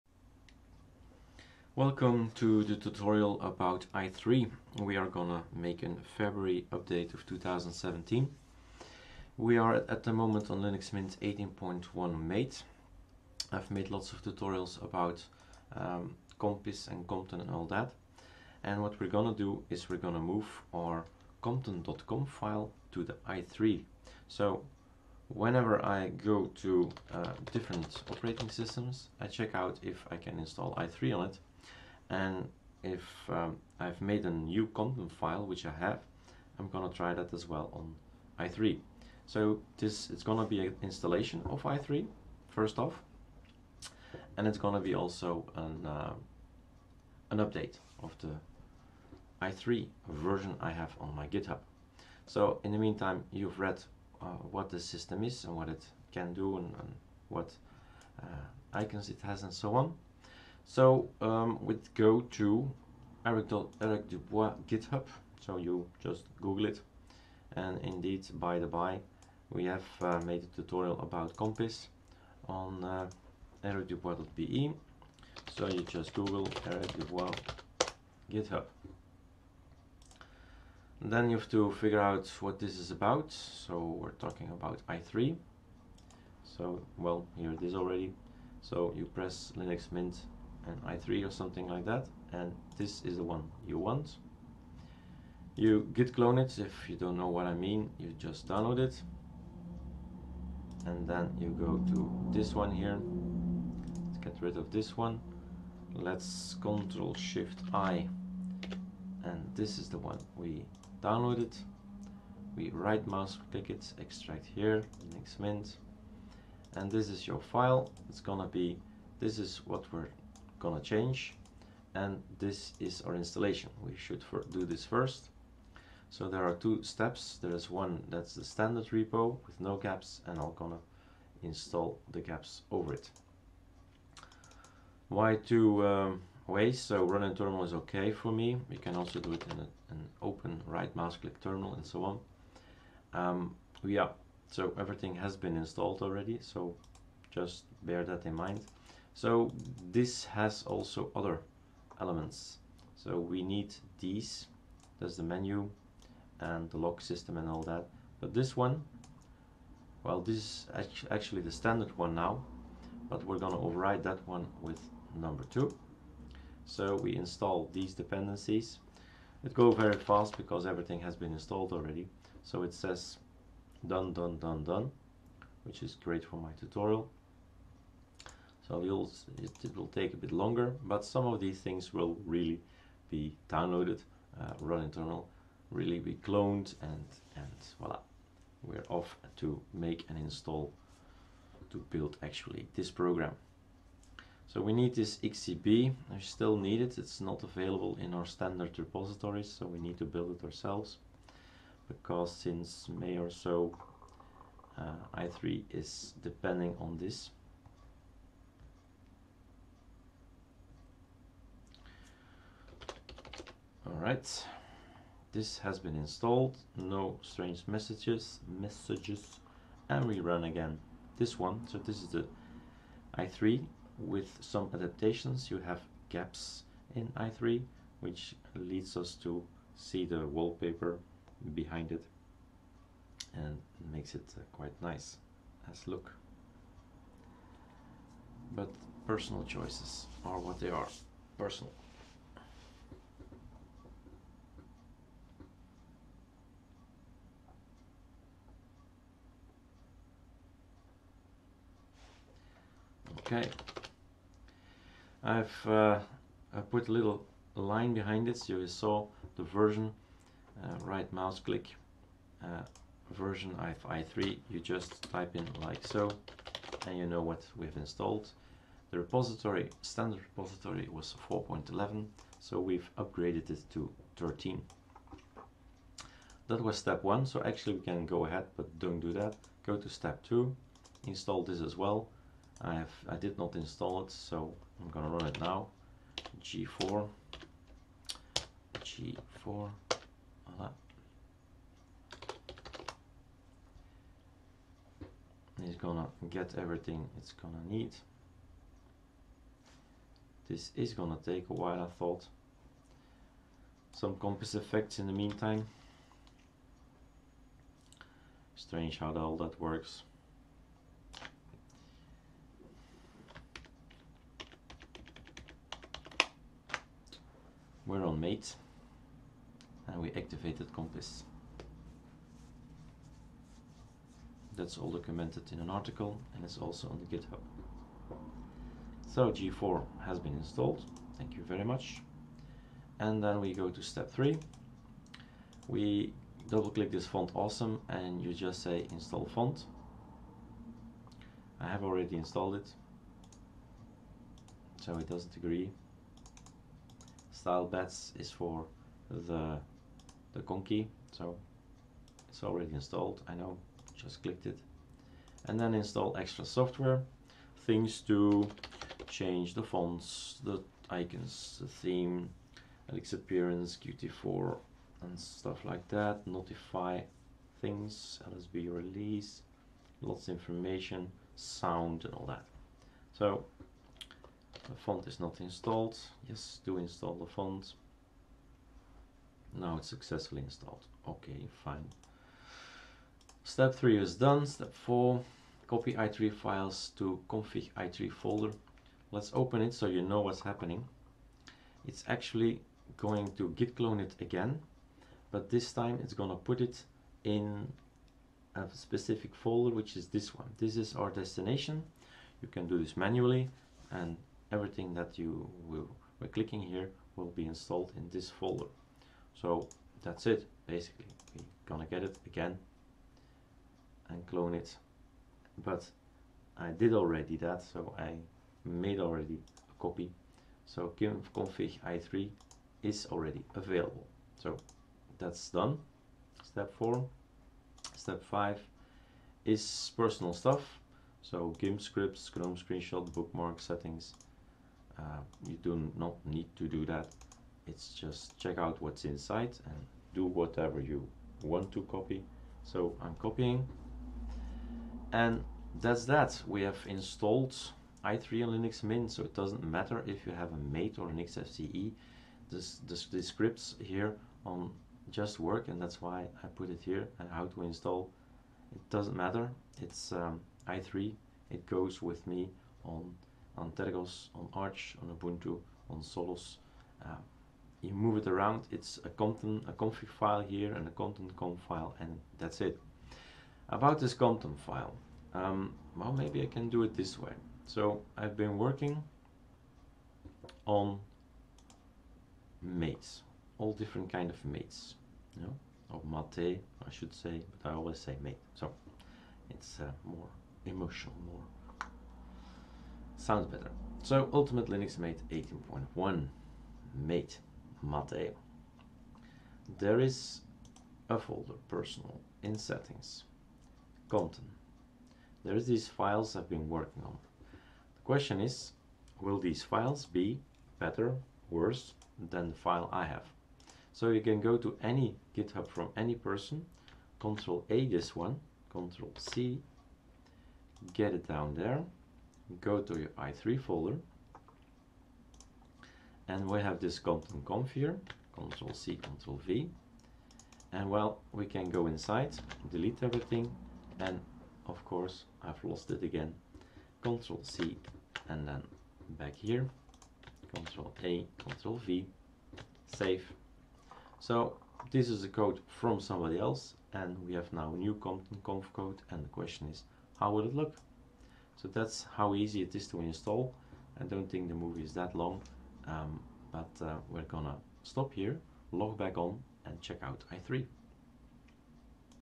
How to install i3 on Linux Mint 18.1 Mate and use compton to have transparency, fading, shadows
We have chosen to install i3wm gaps on Linux Mint 18.1 Mate and use the same compton.conf file for i3.